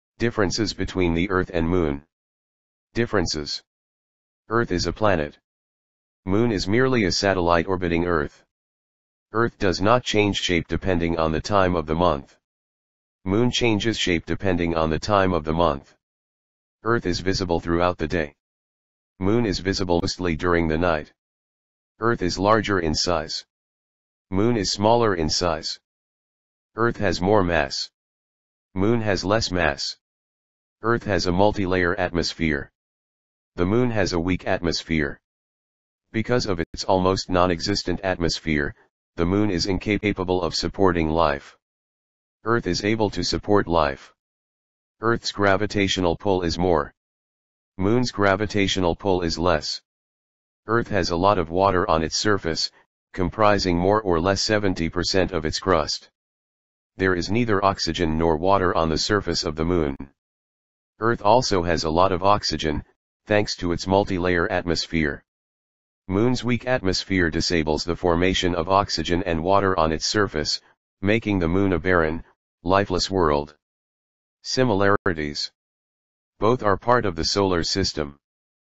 Differences of Earth and Moon
Earth is the first planet from the Sun to have a moon. Despite being Earth's closest neighbor, the Moon is very different from the Earth. It has no atmosphere and the surface is billions of years old. The dark areas in the image below are called maria.Even though the moon is much smaller than the sun, it has two times more influence on tides than the sun, simply because it is much closer to the Earth. The moon's gravitational force pulls on water in the oceans and causes bulges that create “high tide."There are many differences: The first major difference between the two heavenly bodies is that the Earth is a planet, while the moon is merely a satellite orbiting Earth. All satellites are smaller in diameter when compared to planets, and they also have different atmosphere and surface conditions.At its closest approach, Mars is about 50 million km from Earth; at its farthest almost 402 million km (when Earth and Mars are on opposite sides of the Sun). The average distance between Earth and Mars is over 200 times as far as it is from the Earth to the Moon.